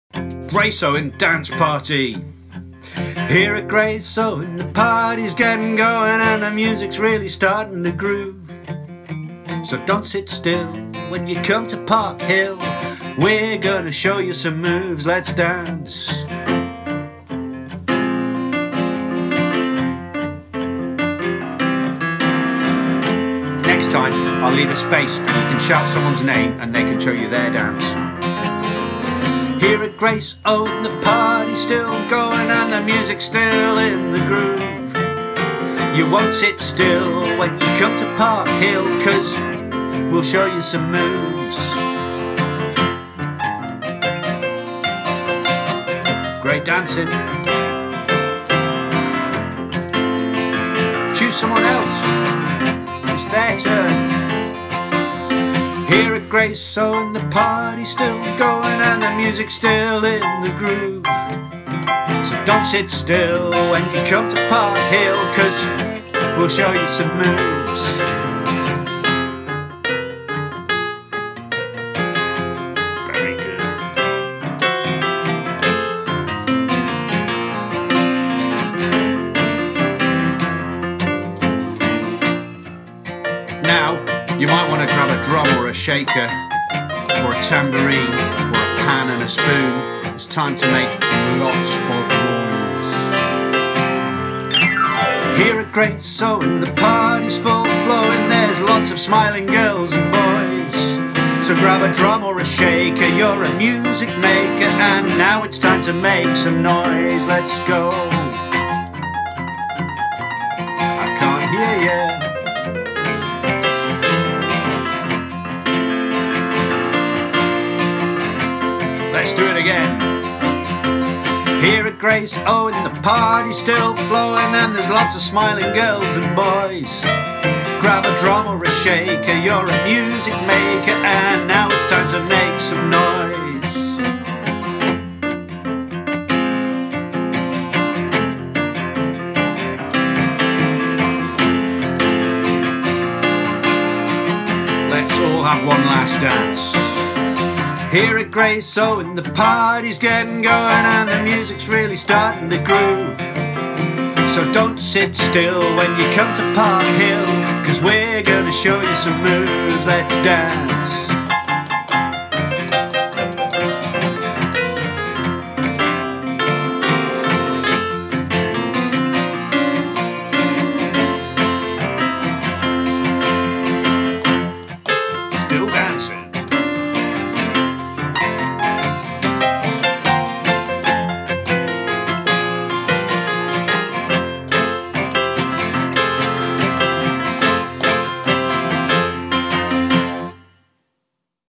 Grace Owen Dance Party!
This is MARTIN HARWOOD singing one of the original songs written on the OUR STREET OUR SONG project with the children from GRACE OWEN NURSERY SCHOOL.
OUR STREET OUR SONG is a 2-year Early Years Music Project with musicians LUKE CARVER GOSS, MARTIN HARWOOD, MOONY WAINWRIGHT and POLLY IVES working with children in 18 Nurseries across South East Sheffield.   Musicians and staff worked with children and families to create their own music and songs using inspiration from their surroundings and heritage.
The project is funded by YOUTH MUSIC and SSELP (Sheffield South East Learning Partnership)